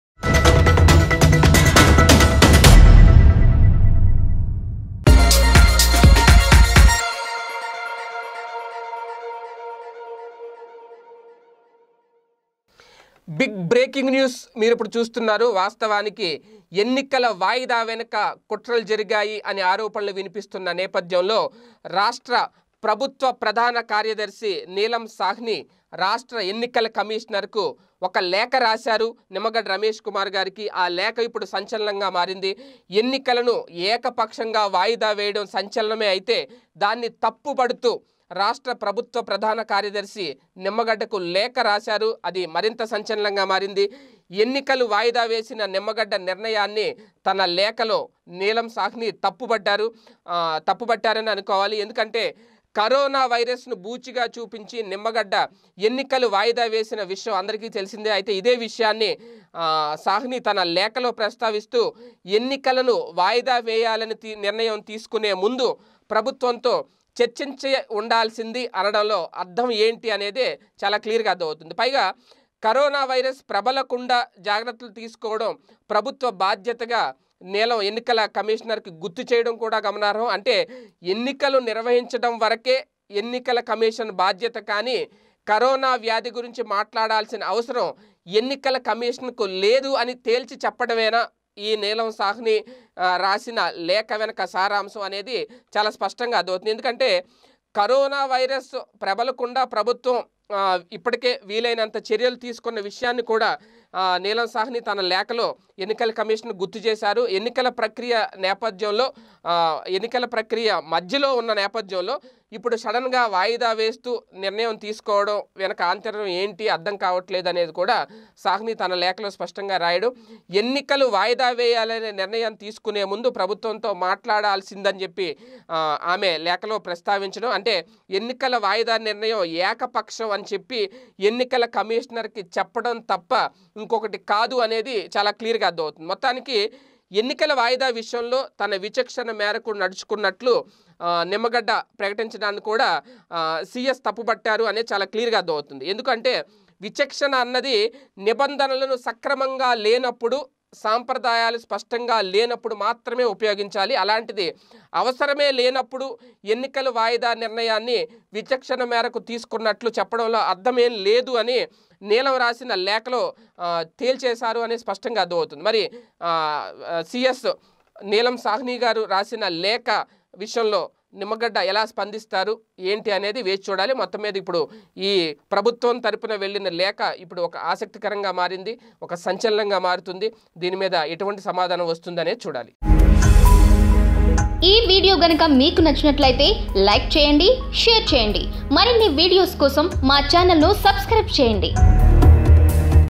AP CS Neelam Sahni Write a Letter to EC | బిగ్ బ్రేకింగ్ : ఎన్నికలు యధాతథం..! సీఎస్ లేఖ
బిగ్ బ్రేకింగ్ : ఎన్నికలు యధాతథం..! సీఎస్ లేఖ | AP CS Neelam Sahni Write a Letter to EC | Local Body Elections | PDTV News